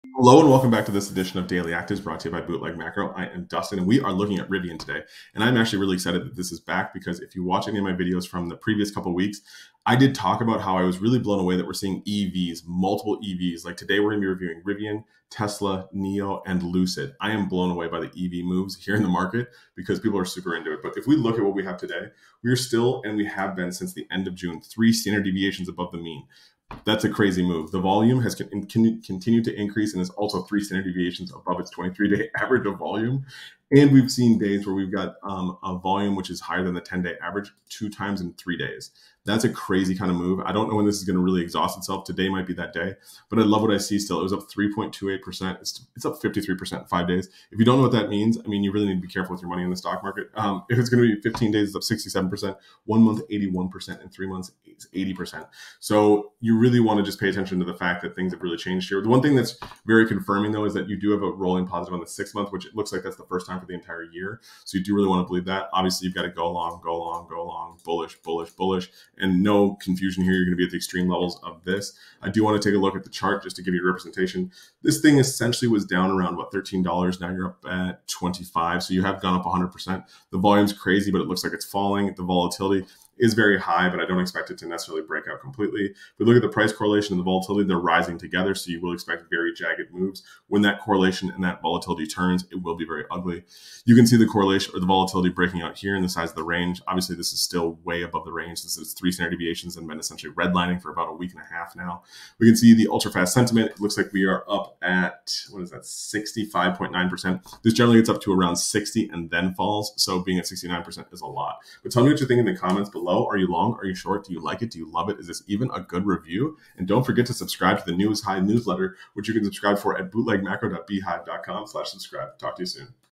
Rivian Review and Market Analysis 🚀 | The EV Market's New Star
Join the New High Newsletter. We send traders 25+ stocks breaking out every week.

In this video, I review Rivian and discuss the recent surge in the EV market. The stock has seen a significant increase in volume and is three standard deviations above its average. Despite the high volatility, the stock has been performing exceptionally well, with a 53% increase in just five days. I will also analyze the chart and highlight the potential risks and opportunities.

In this video, you will learn:

The recent performance of Rivian
The reasons for the surge in the EV market
The risks and opportunities of investing in Rivian
The technical analysis of Rivian's chart
Whether you are long or short on Rivian, this video is essential viewing for anyone who wants to stay up-to-date on the stock's performance.